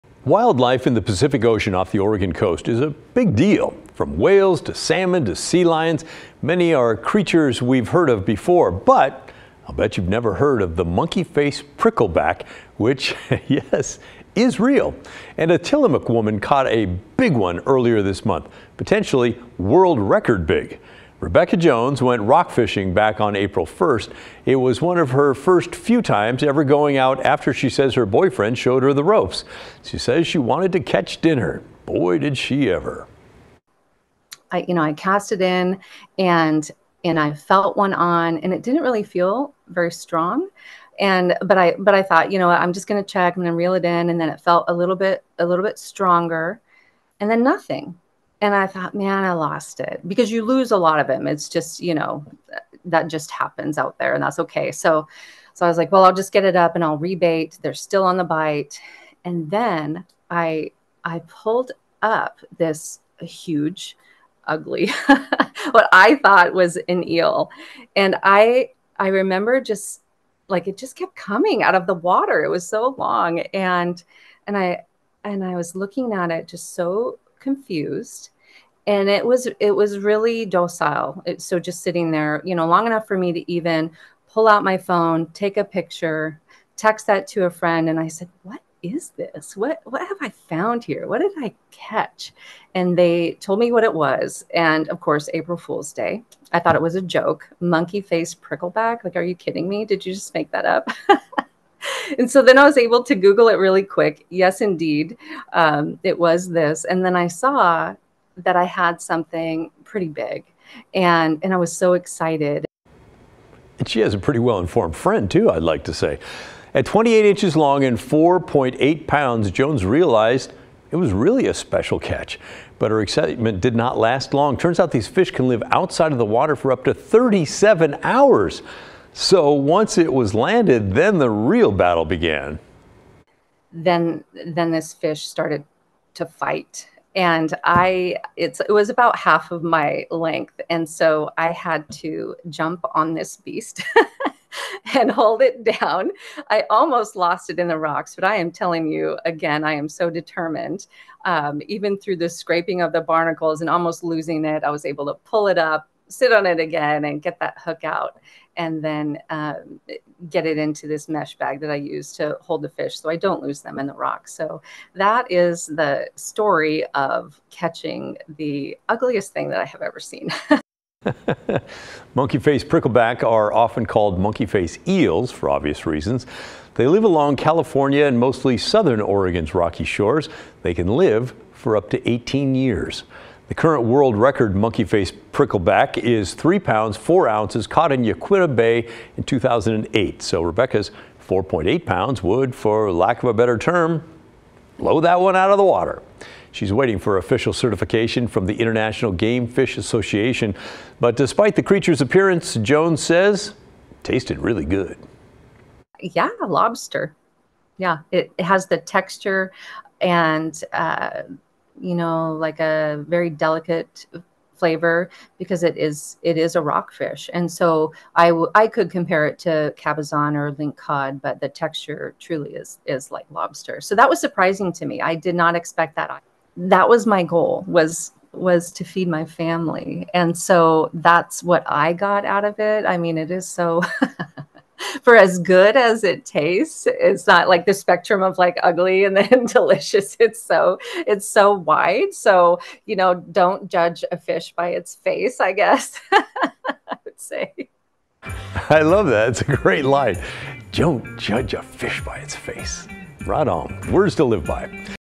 Tillamook woman reels in a monster of a monkeyface prickleback
Rebecca Jones has a reel big fish story after her recent catch of the eel-like rockfish, known as a monkeyface prickleback, which could be a new world record if confirmed. And according to Jones, it was delicious.